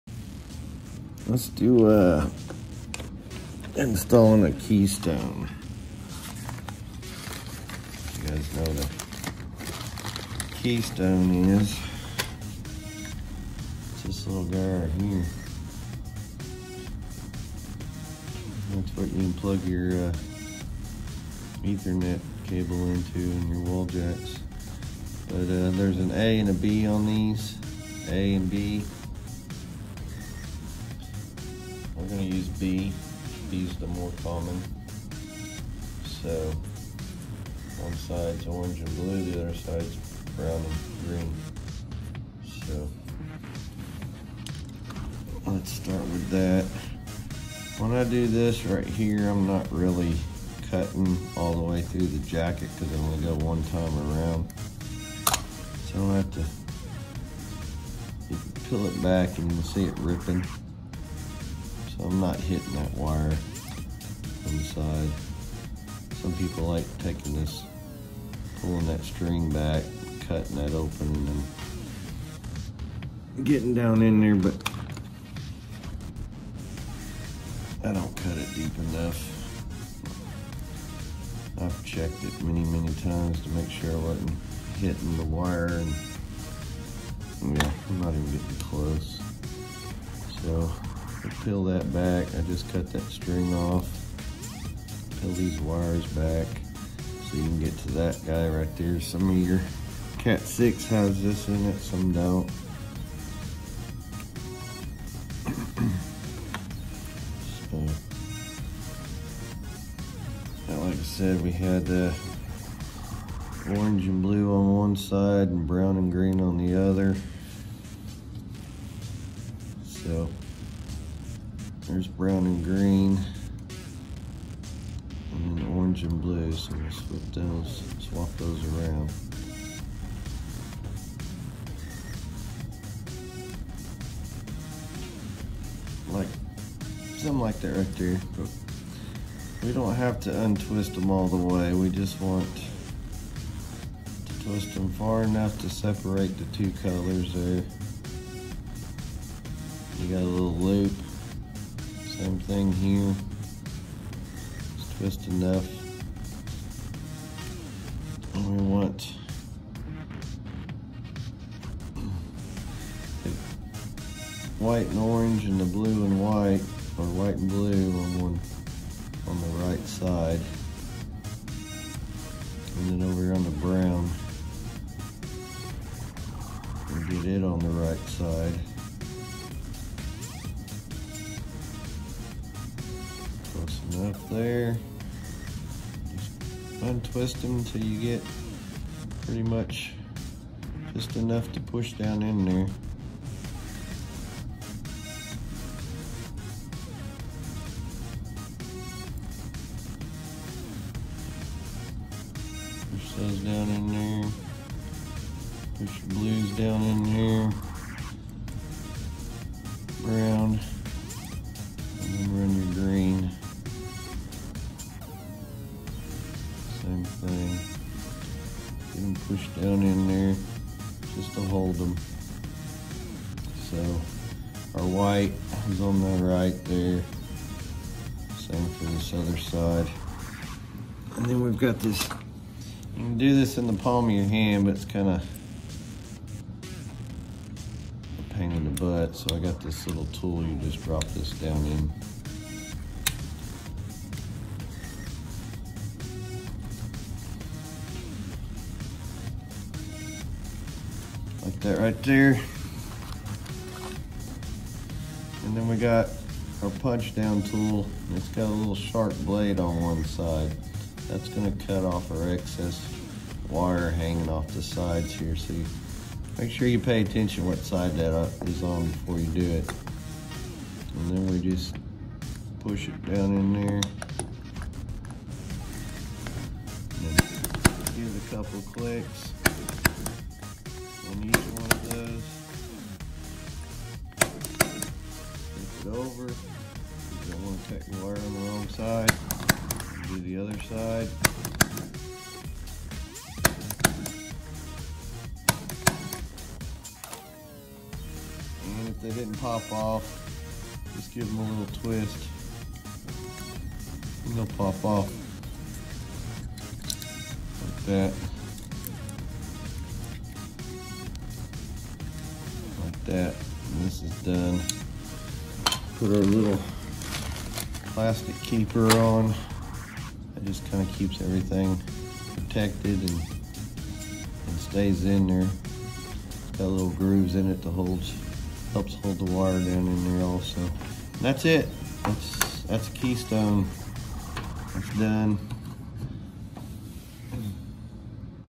Installing an Cat 5 or 6 Wall jack {Keystone}
I’m installing some ethernet keystones for this remodel for our little office. We’re going to have six desk or more in here.